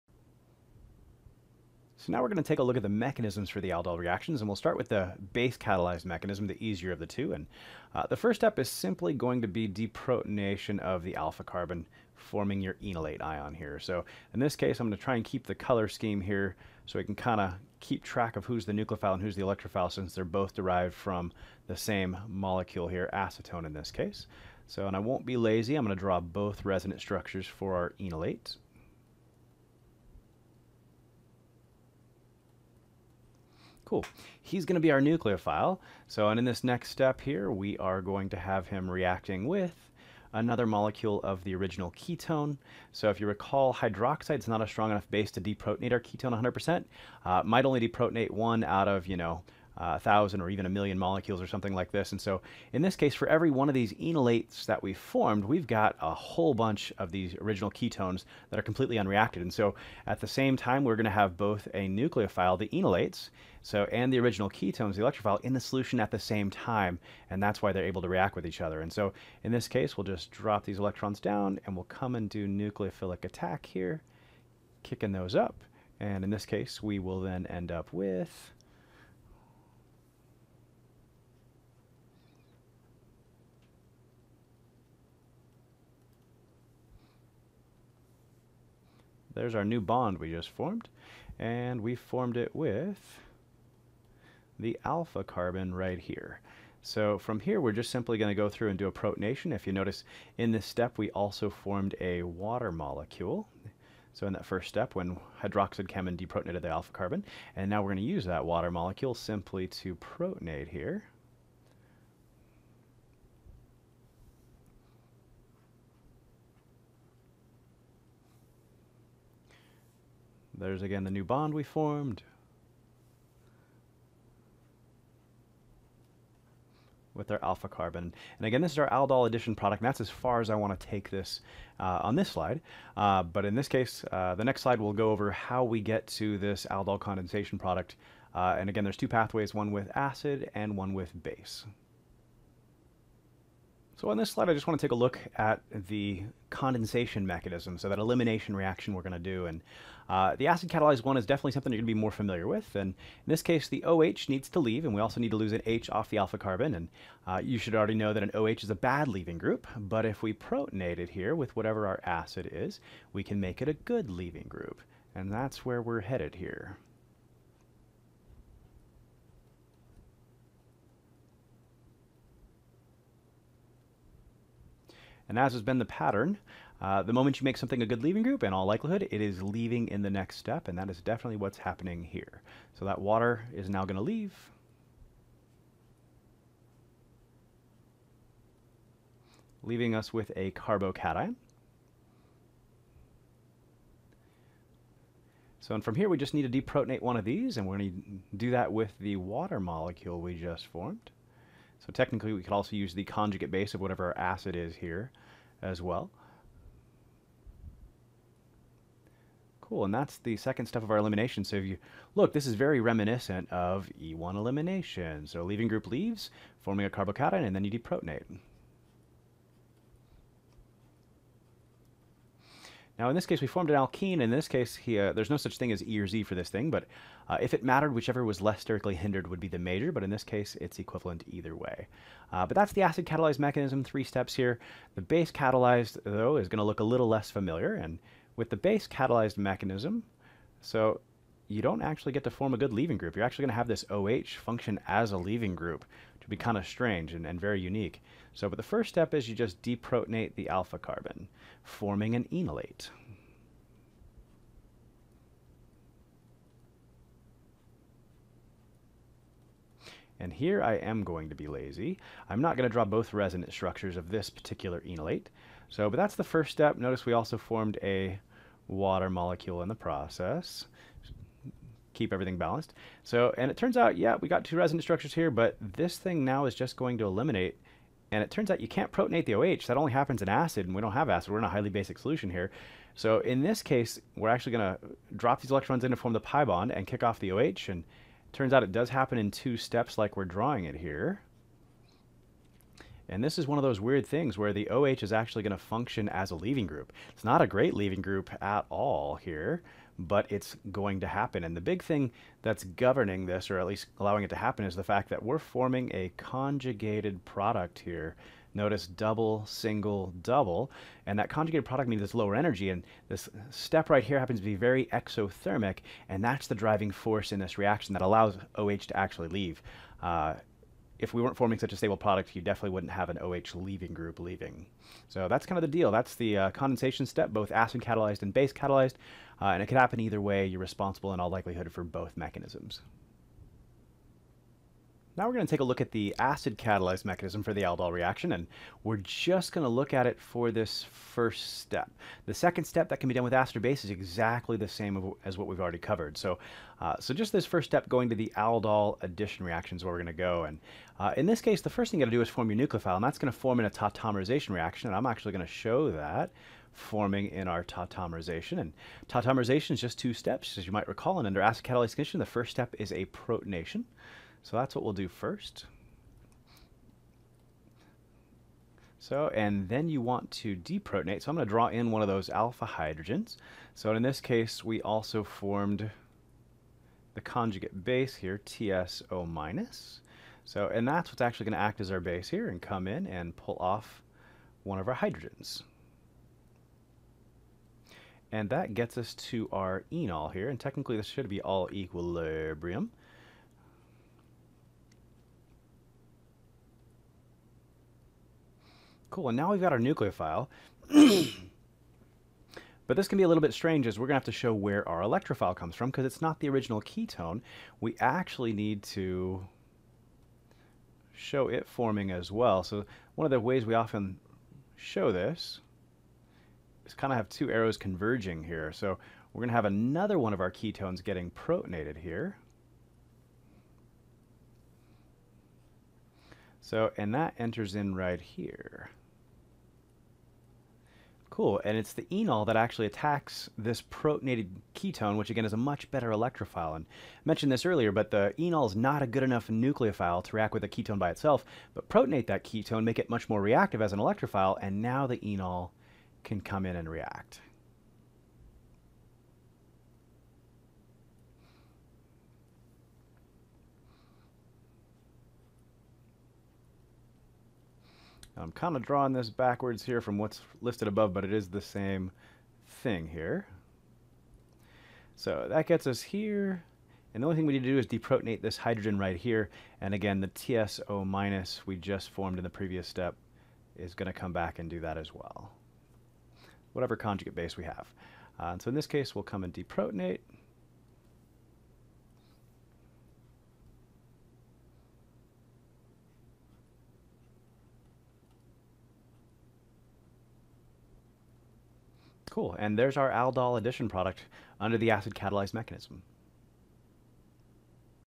21.5b Mechanisms of Aldol Reactions
Chad breaks down both the Base-Catalyzed and Acid-Catalyzed mechanisms for the Aldol Addition and Aldol Condensation reactions in a Self-Aldol reaction.

Happy Studying!